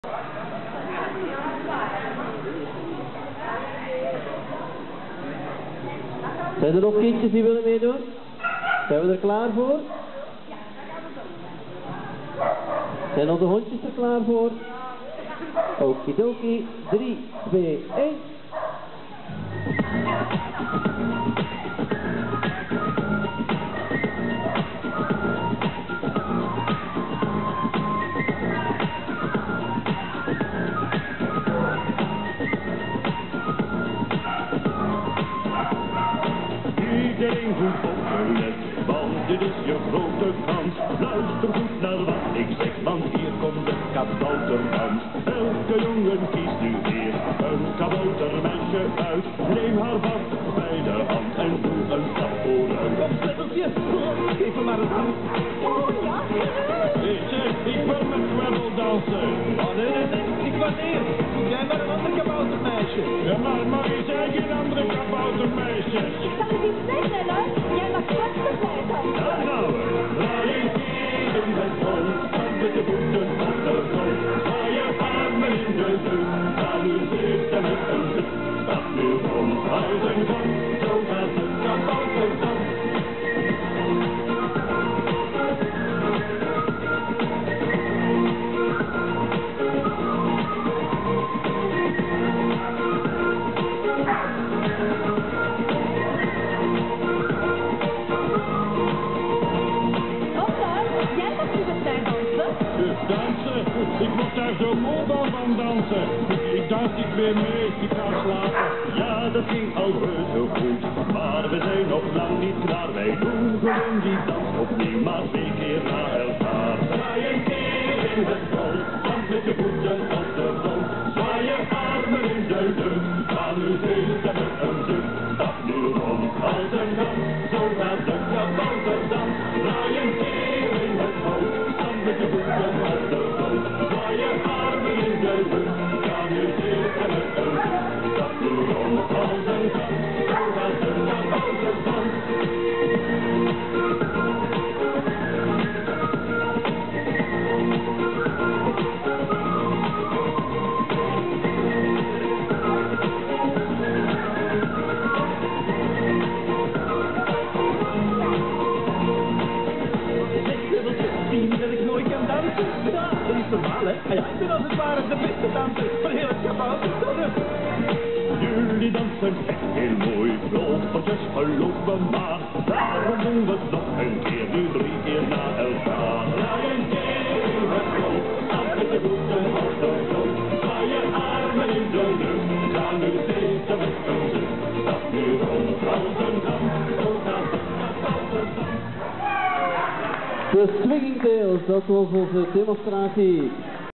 20150501 Doggydance hondendag Ranst - demo 3 plop met cleo yuuka jupi wicca lex plus
Doggydance op de hondendag in Ranst, vrijdag 1 mei 2015 - demo 3 plopdans met cleo yuuka jupi wicca lex en plus